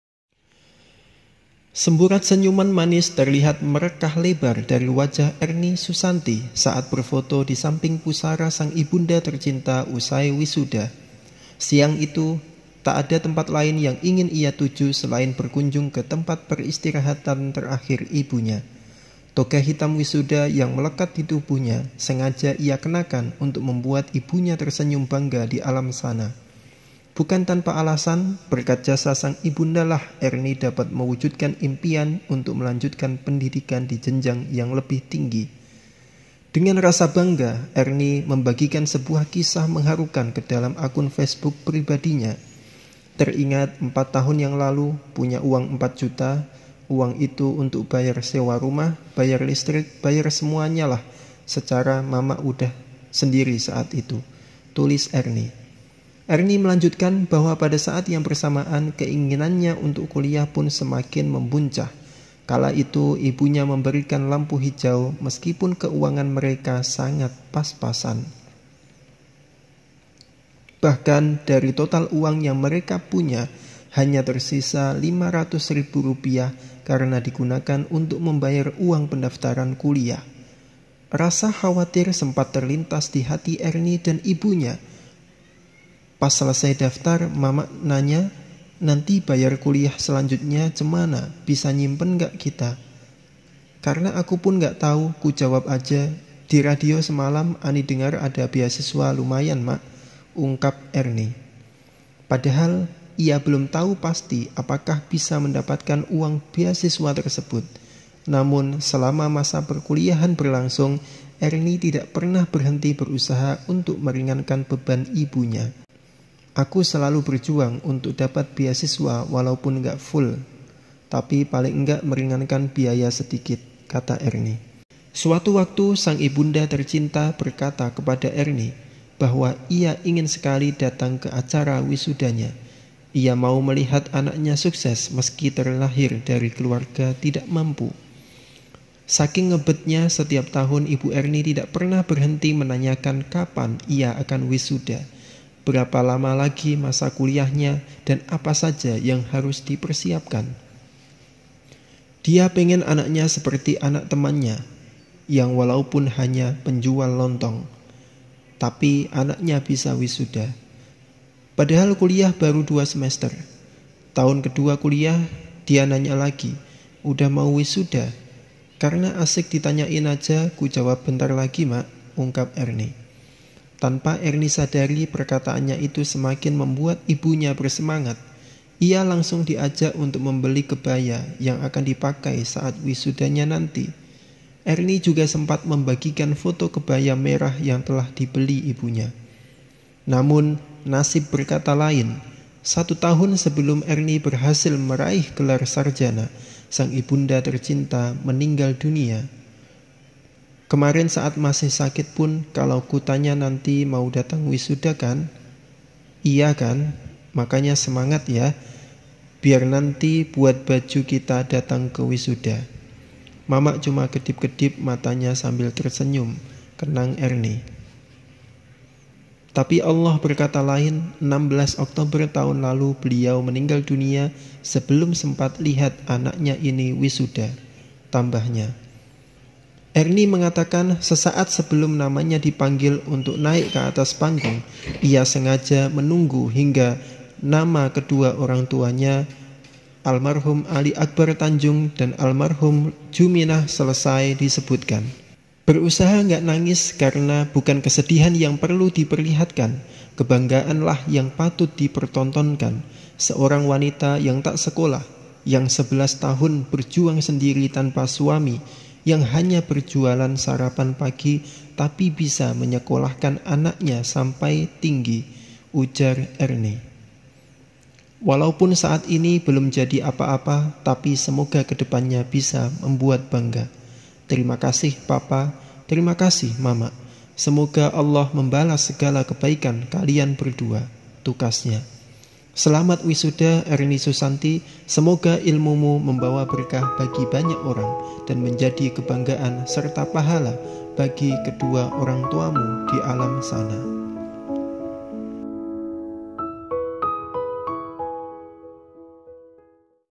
Bikin Nangis | Ibu Berjualan Sarapan 11 Tahun Demi Biaya Sekolah dan Kuliah Anaknya, Sayangnya...
Cerita ini Bikin Nangis. Seorang Ibu Berjualan Sarapan 11 Tahun Demi Biaya Sekolah dan Kuliah Anaknya, Sayangnya